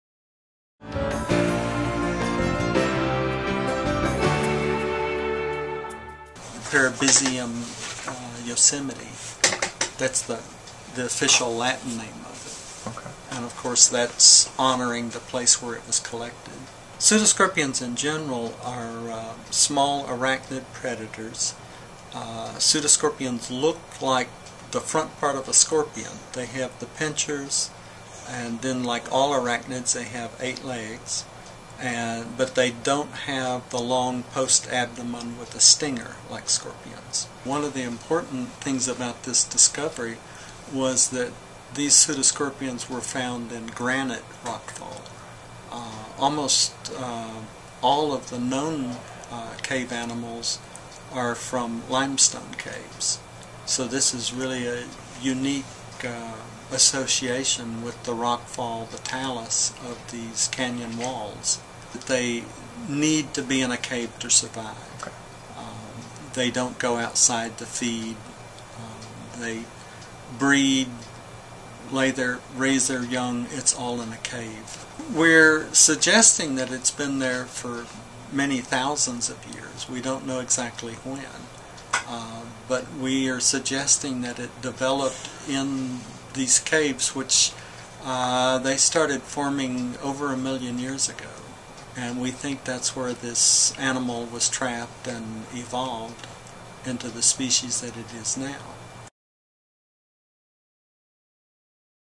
Texas Tech Researcher Documents New Arachnid
Texas Tech researcher James Cokendolpher, a research scientist and assistant curator of invertebrates at The Museum of Texas Tech University, is investigating a newly discovered arachnid.

Parobisium yosemite was discovered a couple of years ago in a cave in Yosemite National Park.  This cave-dewlling species is considered unique because it is able to live in granite rockfalls instead of limestone caves where most other species live.